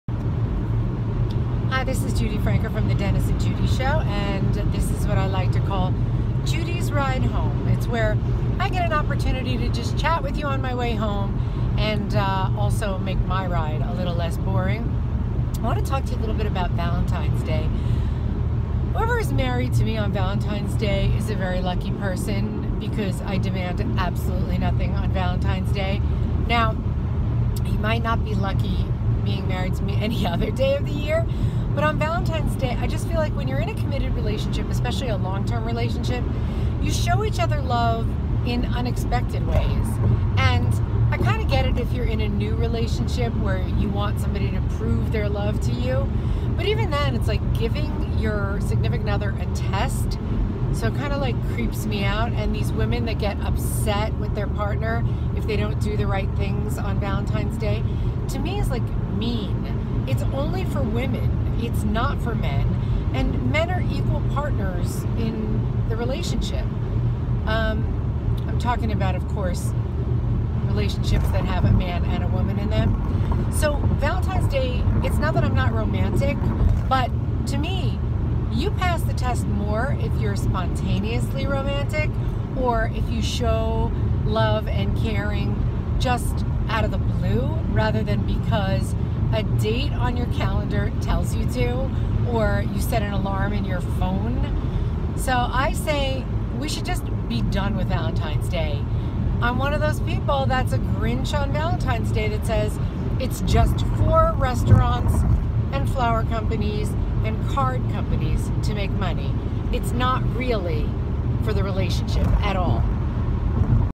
NJ's Valentine's Day 'Grinch?'
During her ride to work, Judi Franco explains why we really need to "just be done with Valentine's Day."